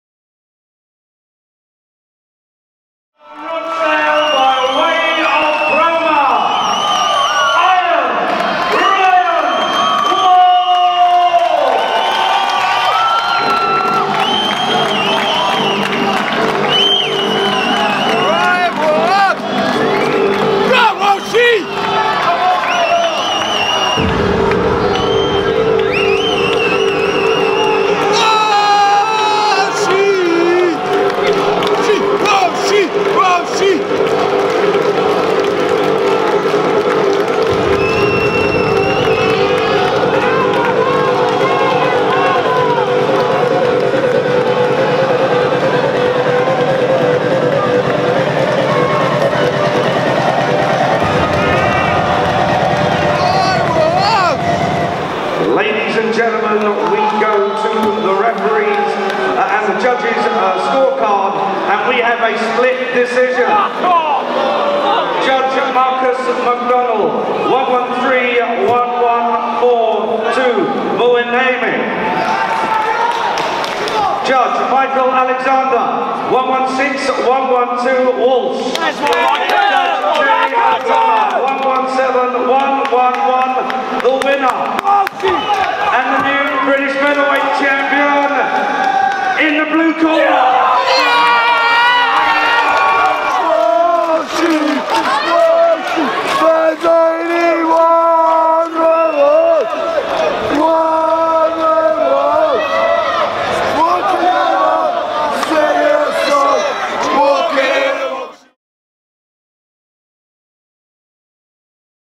Ryan Walsh becoming the new British featherweight champion
The night history was made! WARNING: contains strong language.